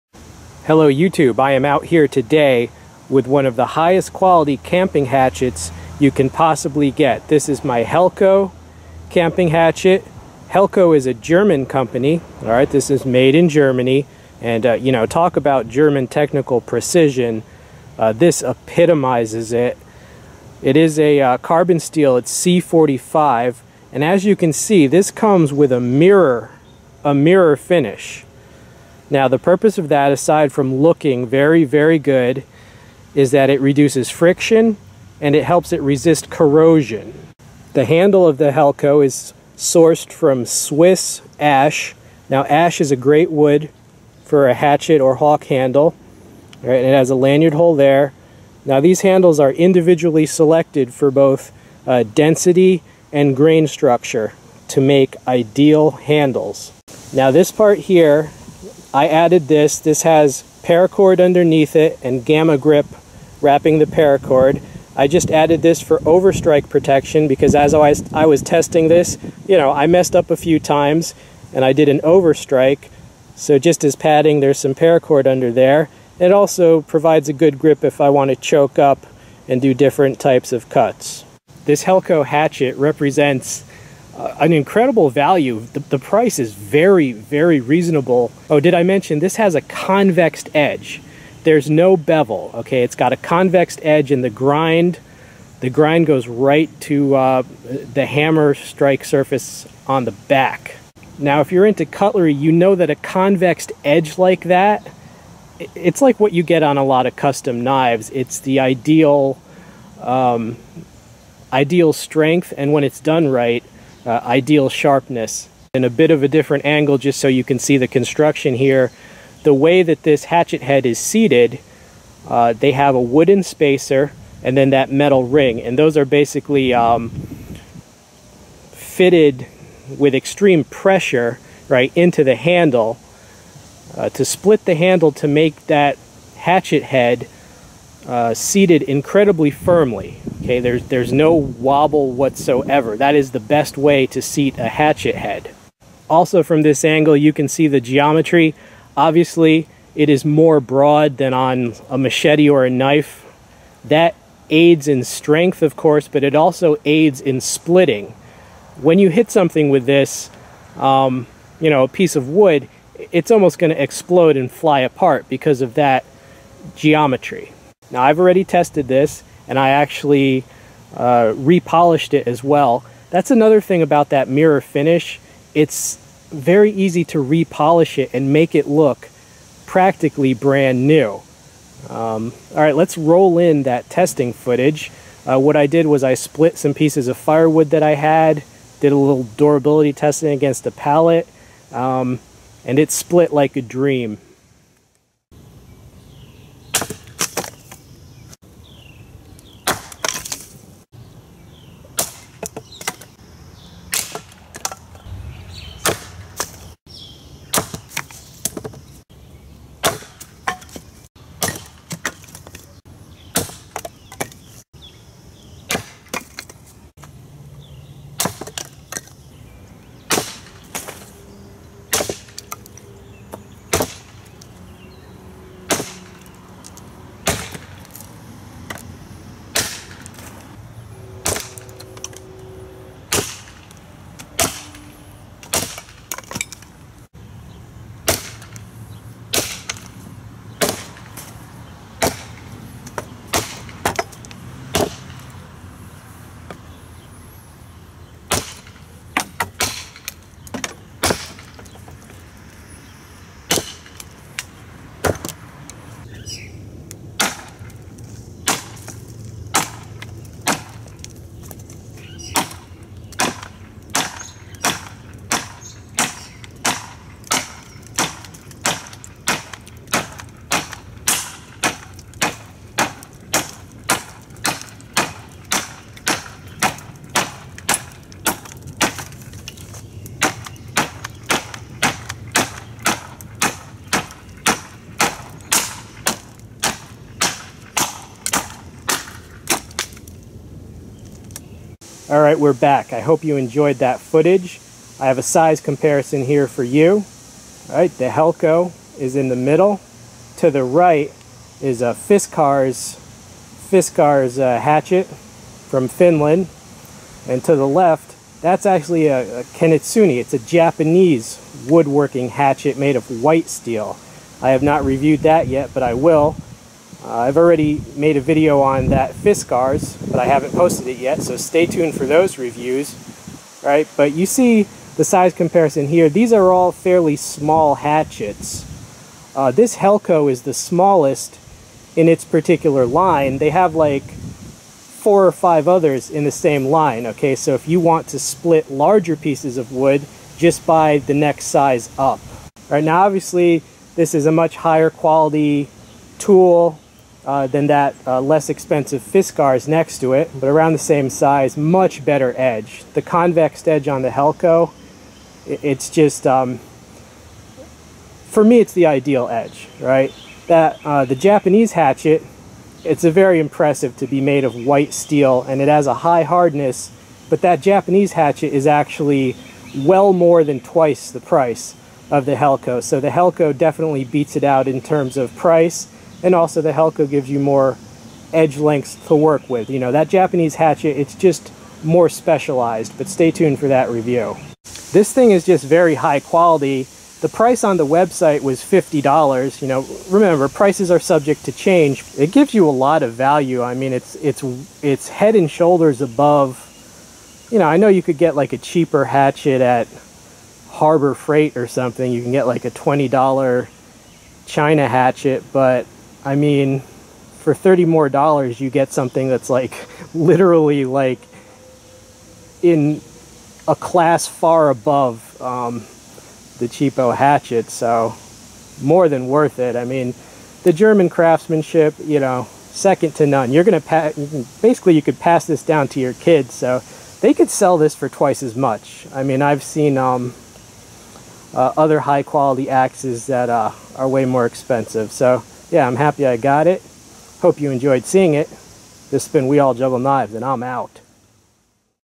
Helko Mirror Finish German Camping Hatchet
Edge Length: 3 inches
Steel: C45 Carbon (53-56 HRC)
Total Length: 14.5 inches
Handle: Wood, Swiss Ash
Weight: 1.6 pounds
Blade Guard: Leather
Made In: Germany

From Helko's Classic Line, this is one of the highest quality camping hatchets one can buy. The 1.1 pound hatchet head is drop forged in Germany. It comes with a mirror polish to reduce friction and resist corrosion. It has a true convex edge that is very strong and sharp. The handle is selected from Swiss Ash for grain and density, then lightly lacquered.